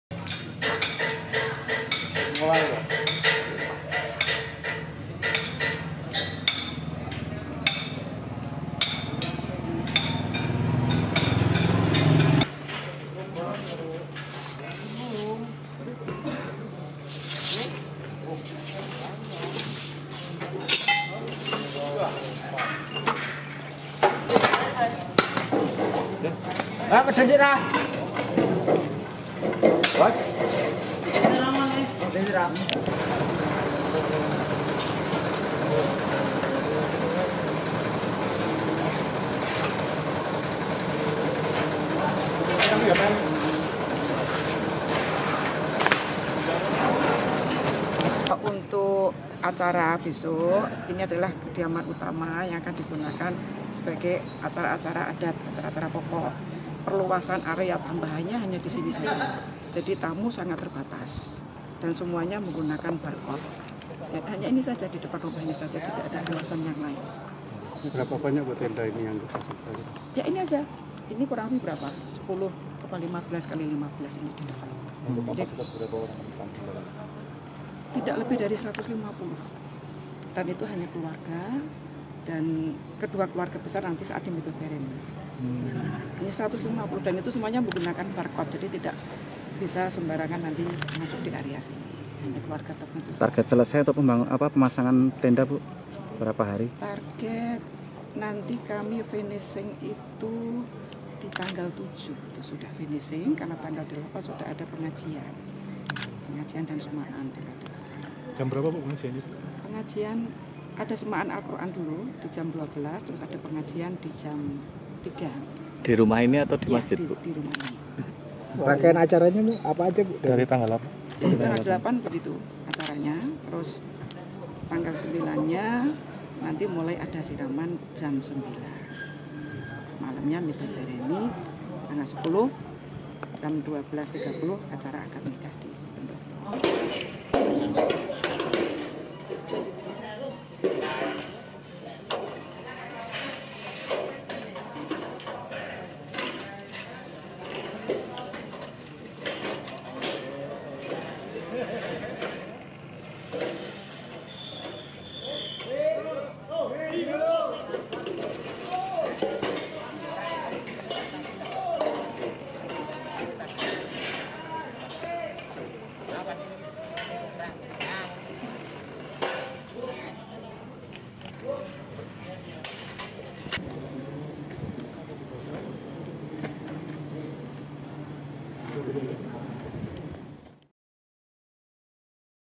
Detail rencana pernikahan Kaesang-Erina, para tamu pakai barcode
Rangkaian acara pernikahan Kaesang Pangarep dan Erina Gudono sudah dimulai di rumah Erina di Purwosari, Kalurahan Sinduadi, Kapanewon Mlati, Sleman. Tenda di depan rumah Erina dan sejumlah CCTV sudah mulai dipasang pada Kamis (1/12/2022).

CEO Pengantin Production, Dani Wigung, menjelaskan rumah Erina akan digunakan untuk sejumlah acara adat dengan menggunakan sepetak tanah kosong di depan rumah.

Tanah kosong itu seluas sekitar 15X15 meter, yang saat ini sudah mulai didirikan tenda. Estimasi tamu yang akan berada di tempat ini sekitar 150 dari keluarga kedua calon mempelai.

“Semuanya menggunakan barcode jadi tidak semua orang bisa sembarangan masuk ke area sini,” katanya, Kamis (1/12/2022).

Video: Harian Jogja/Gigih M. Hanafi

untuk bisa mendapatkan perkembangan berita dan peristiwa terbaru di sekitar kita.